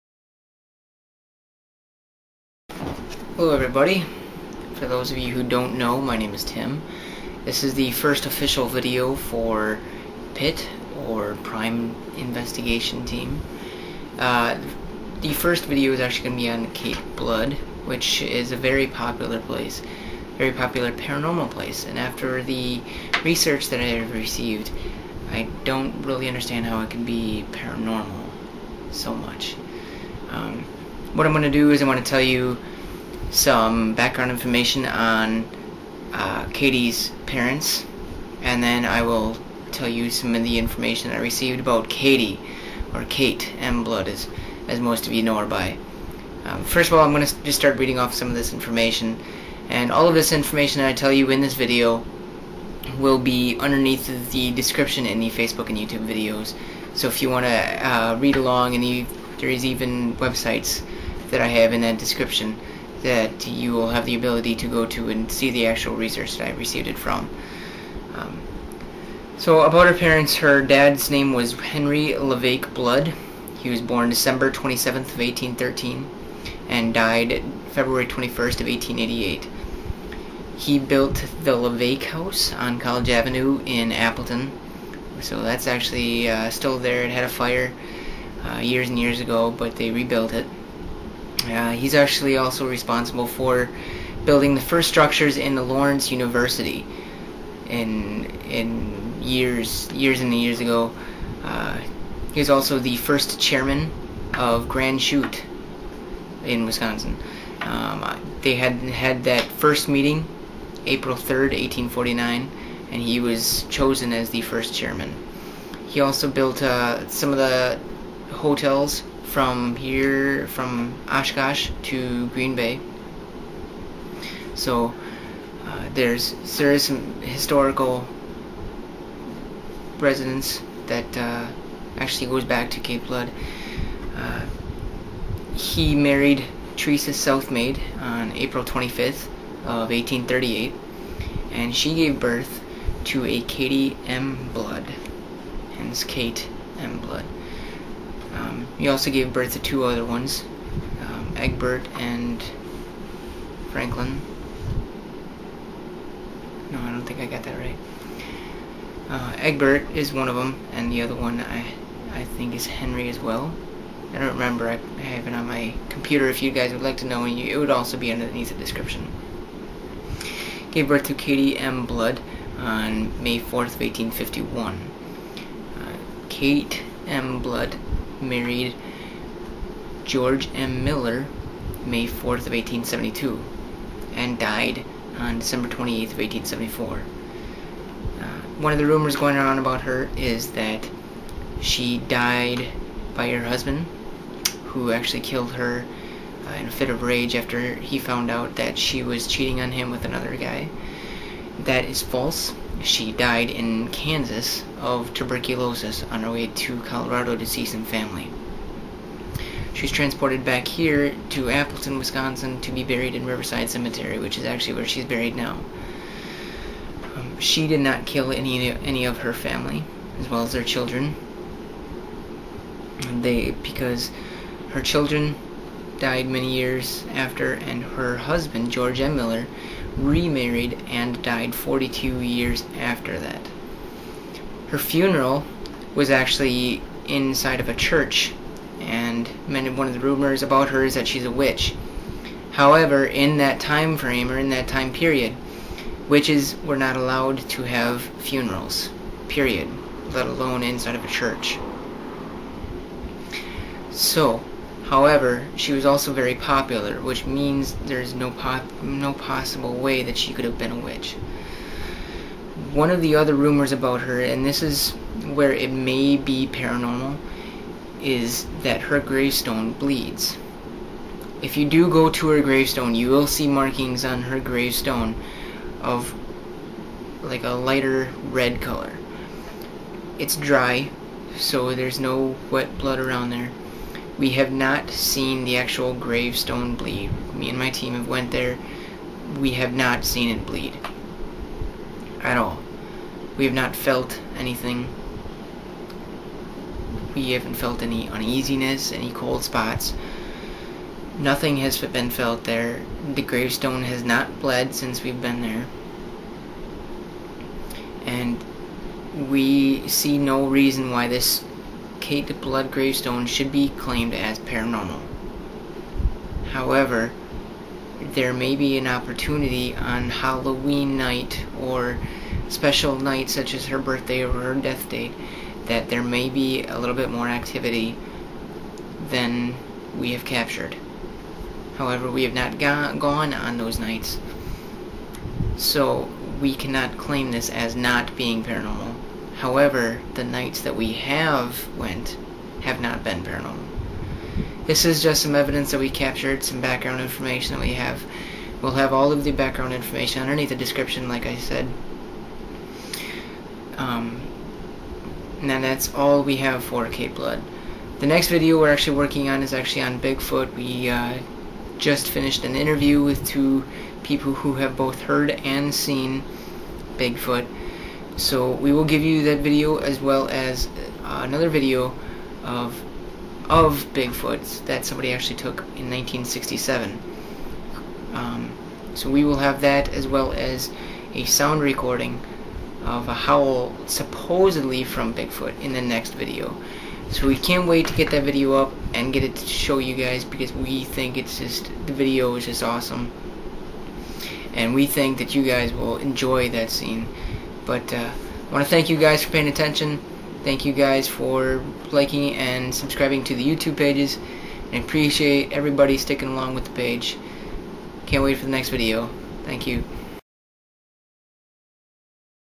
Kate Blood
Kate M. Blood (Miller) (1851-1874)

Blood was born and raised in Appleton, Wisconsin. She was loved by many as a mother, daughter, and wife. Many folk-lore have been created about her in recent times; most of which can be proven illegitimate.

The Myths
The many stories that are known are probably caused from the fact that her name is Blood and that the location of her grave is separated from the rest at Riverside Cemetery in Appleton.

(A) It has been told that she murdered her husband, three children, and then committed suicide. Thus the officials of the city place their grave site away from the others to hide her deed.

(B) "The true story is that her husband killed her in a drunken rage believing she was sleeping with one of her male friends, when in all actually she was only talking to him about her problems with her husband. I don't quite know what happened to the friend, but I don't quite think she realized the seriousness of the situation till the very end."

The Truths
(A) George M. Miller (Husband) did not die until 1916; that's 42 years after Kate's death, therefore she could not have killed him. The initials on the three small tombstones (portrayed as the three children) near a large one actually can be compared to the names on the large one; it is the same people: Kate, Kate's husband, and Kate's husband's second wife. (Often not seen since it is on the side)

(B) This story is some-what more believable, yet still false due to research. She actually died from consumption (Tuberculosis) in Kansas visiting relatives on her way to Colorado.

There is the evidence of logic, however. In an obituary it states that the funeral might be held in a church, which many would think why would they want a witch's funeral in their church. She was quite popular, which most witches in that day were not.

"Generation NO. 7

Henry Laveque Blood (Giles, ASA, Nathaniel, Richard, Joseph, Richard) was born December 27, 1813 in Orford, Grafton CO., NH, and died February 21, 1888 in Appleton, Outagamie CO., WI. He married Theresa Catherine Southmayd April 25, 1838 in Jay, NY, daughter of Nathan Southmayd and Catherine Southmayd.

Children of Henry Blood and Theresa Southmayd are:

1. Egbert J. Blood born April 10, 1846, Mackinaw, MI; died September 27, 1885, Appleton, Outagamie CO., WI; married Laura D. Washburn between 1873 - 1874, Red Wing, MN.

2. Frederick Henry Blood born August 06, 1848, Green Bay, WI; died February 11, 1905, Appleton, WI.

3. Katie (Kitty) M. Blood born May 04, 1851, WI; died December 28, 1874; married George M. Miller, May 4, 1872."

"Pioneer and businessman, helped lay out the plat for Appleton in 1848; became the first town chairman of Grand Chute while it was still part of Brown County. Built the Levake house, one of Appleton's finest early hotels. Involved with building the first structures of Lawrence University in 1847."

"The first official meeting for the Township of Grand Chute was held in Appleton on April 3, 1849, in the home of W.S. Warner. The first order of business was the election of town officers. Henry Laveque Blood was chosen as town chairman and assessor, Ezra L. Thurber as town clerk, John Stevens as inspector of schools, and Hiram Polly as treasurer and tax collector. In addition, two supervisors were selected, along with three constables, and four justices of the peace. After the election of officers, a budget of $200 was adopted. To raise the money for the budget, a tax of $2.50 was imposed on each quarter section of deeded land. Any landowner unable or unwilling to pay the tax had the option of giving the township two days work instead."

"At the election held April 3, 1849, Henry L, Blood was elected chairman; Julius S, Buck and William H, McGregor, supervisors; Julius S. Buck, Robert R. Bateman, Bela B. Murch and Samuel P. Blake, justices of the peace; Ezra L. Thurber, town clerk; Henry L. Blood, assessor."